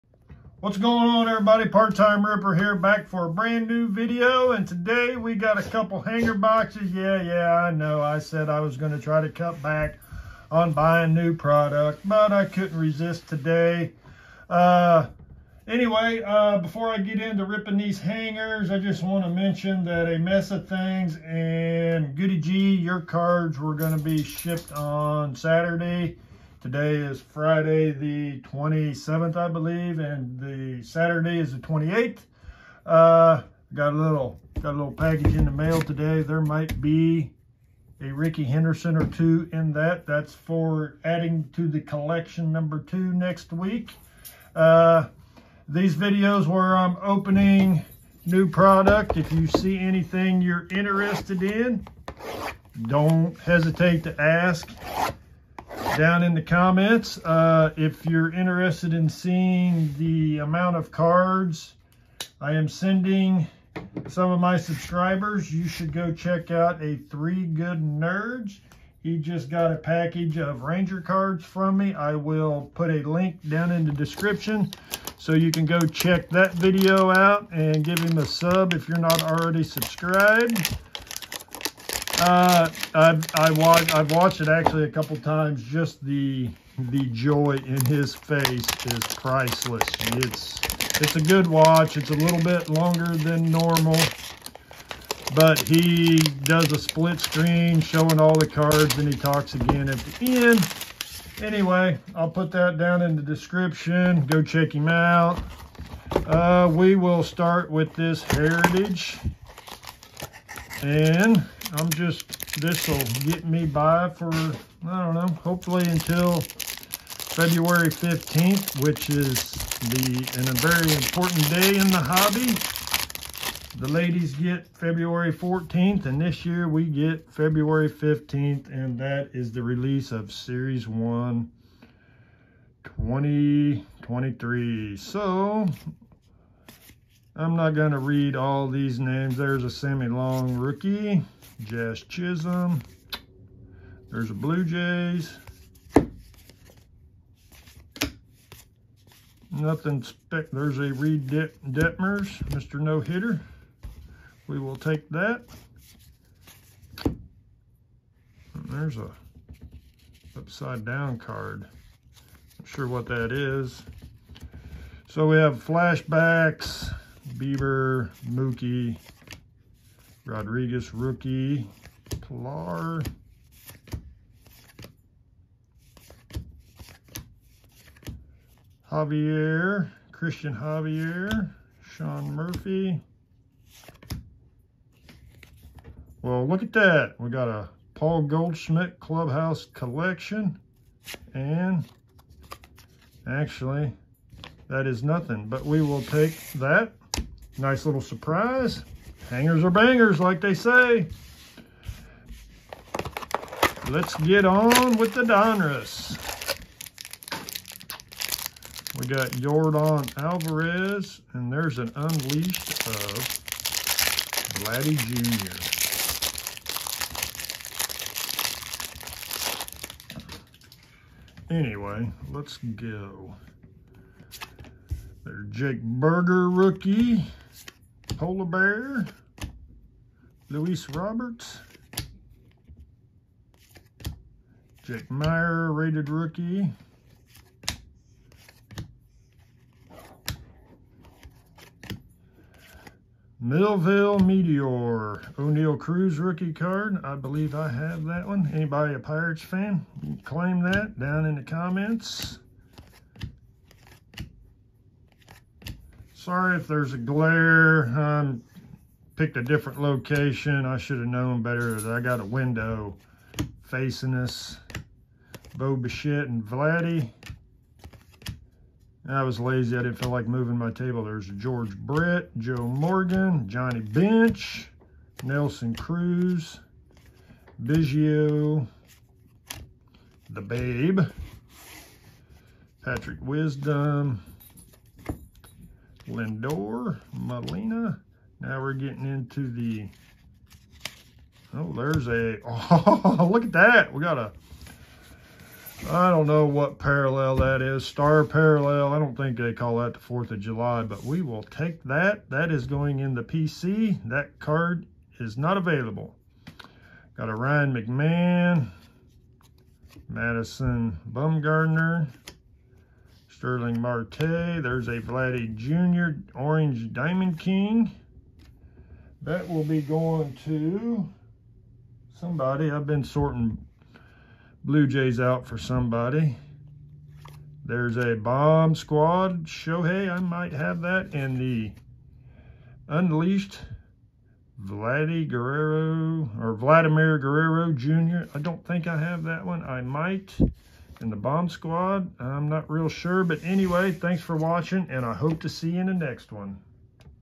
Ripping 2 Hanger Boxes : Huge Rookie Parallel
I just felt the urge to rip open some cards.
If you saw anything that you liked don't hesitate to ask about them in the comments below.